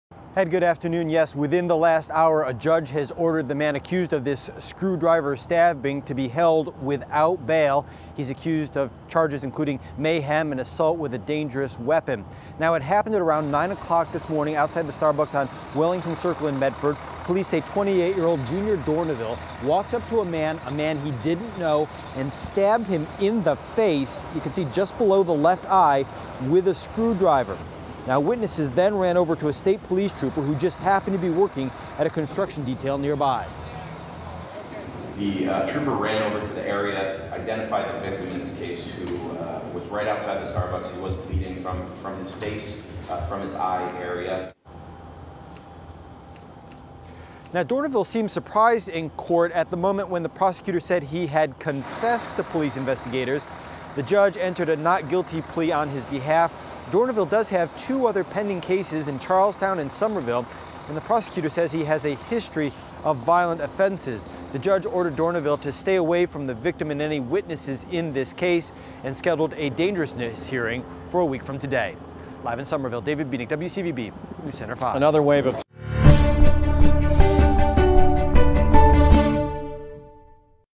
Screwdriver used in random attack outside Medford Starbucks
A man was hospitalized Tuesday after being attacked with a screwdriver outside a Starbucks in Medford.